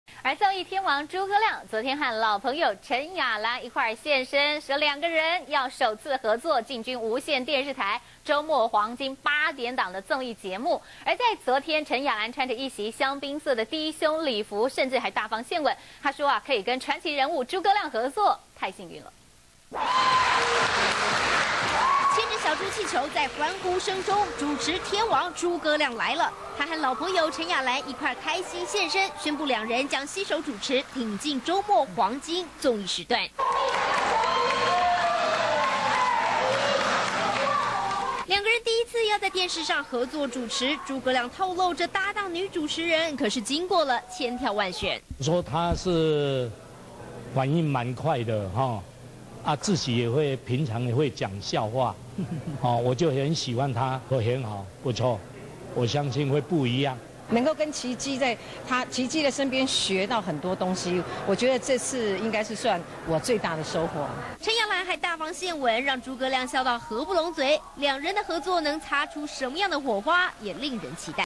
豬哥亮陳亞蘭聯手 進軍週末黃金綜藝
中視新聞》之前忙著拍電影的綜藝天王豬哥亮，首次入主無線台8點綜藝，搭檔人選原來就是和豬哥亮私交甚篤的陳亞蘭。豬哥亮與陳亞蘭將聯手主持的新綜藝節目《萬秀豬王》，將在8月於中視首播。為呼應節目名稱，兩人牽著可愛豬造型氣球出場，還共飲「巨無霸珍珠奶茶」，代表豬哥亮是中視的「珍珠(豬)寶貝」、陳亞蘭則是「掌上明珠」。